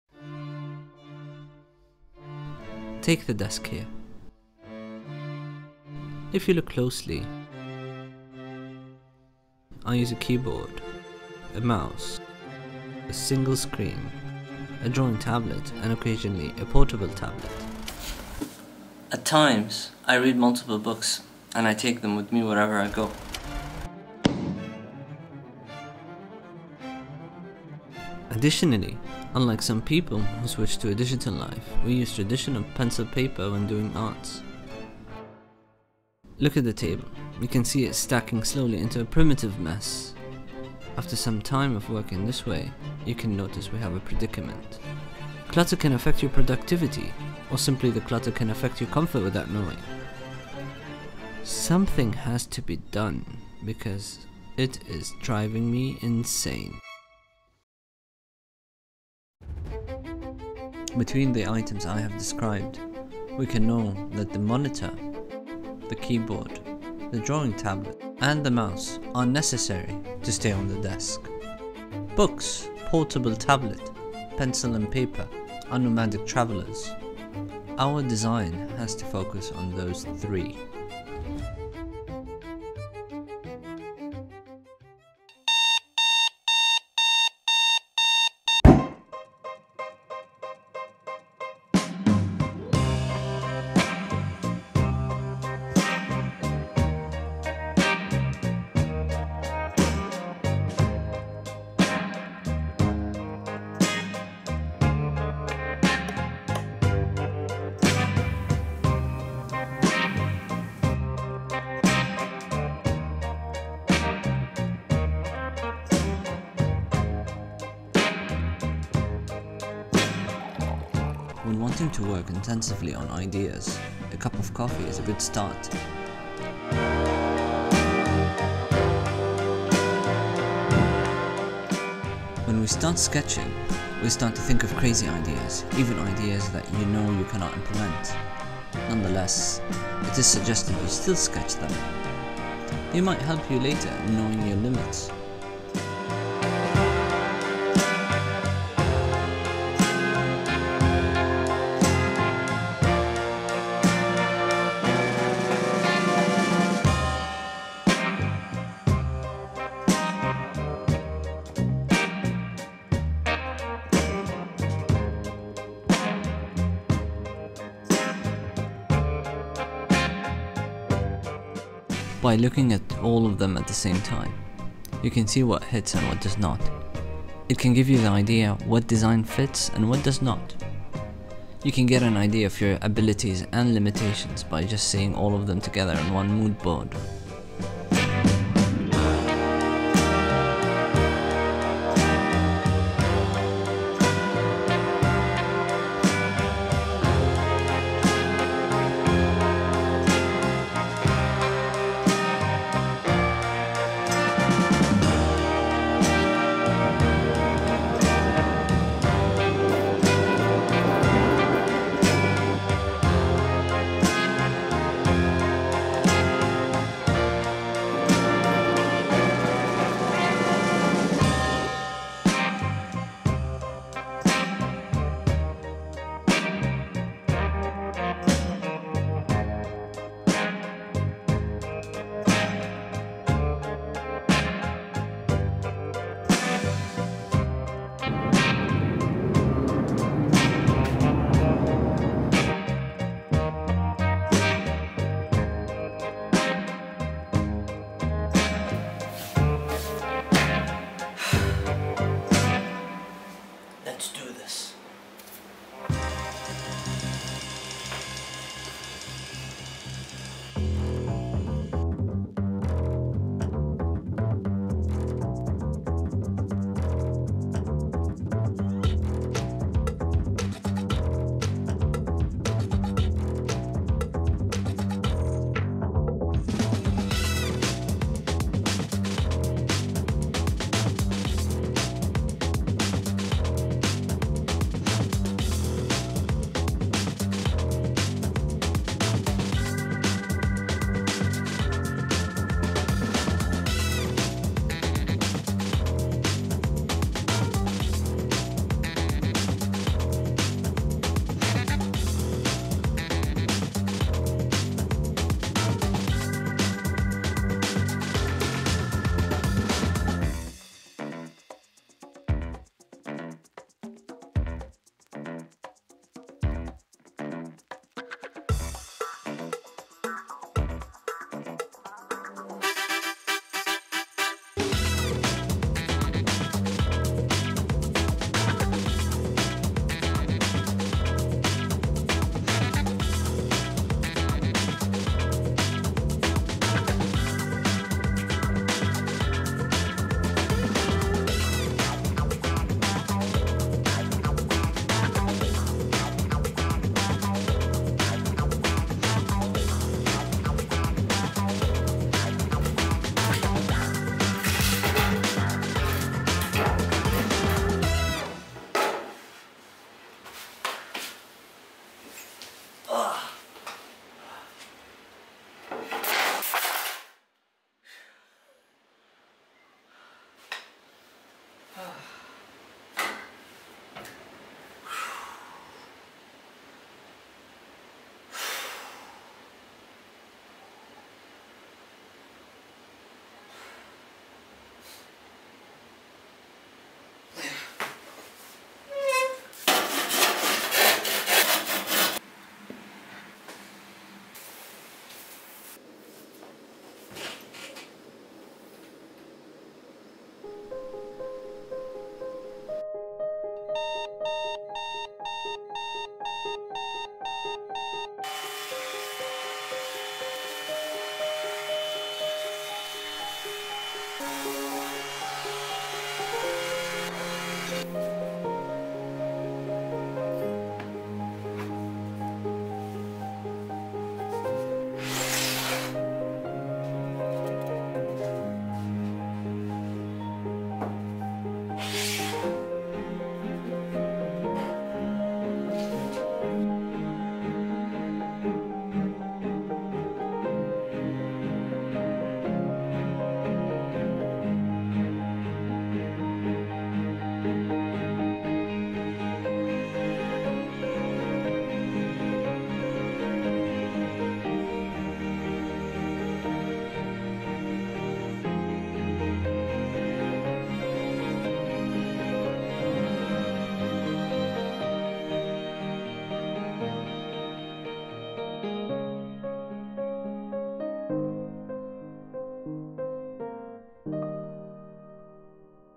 The Table is a Mess...designing for Chaos!
The Office desk is always messy. Our habits only make it worse.
So we designed a holder that can hold our portable products as a resting place until move on from the desk.

This is our first published product design out here. We show how the process was done and the final outcome. The steps include the following:
1. Thinking of the problem.
2. Thinking of a solution to the problem.
3. Sketching the design.
4. Buying the products needed for the design.
5. Build the design.
6. Test and use.

🎼 Music used in the video:
Music from Uppbeat (free for Creators!)

Salam!!